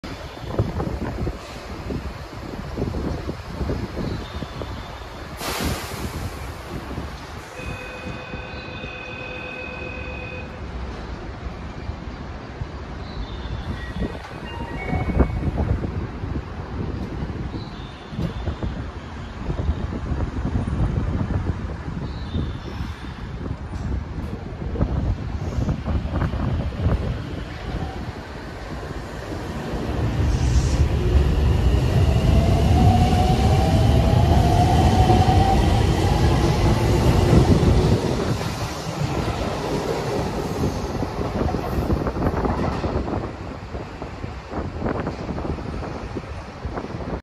あおなみ線野跡駅 Noseki AN10 1101編成 発車シーン (ホーム柵更新前)
ご案内致します。
ささしまライブ駅、1番線では、6月18日から、
ホーム柵更新工事を実施致します。詳しくは、
ホームページ、または、駅のポスターを
ご確認ください。この後の7月20日から、
ささしまライブ駅の名古屋方面の
ホーム柵更新工事を実施致します。工事期間中は、
全ての扉が利用出来ます。
また、列車の各駅発着時刻は、変更致しません。
詳しくは、ホームページ、駅のポスター等を
ご確認ください。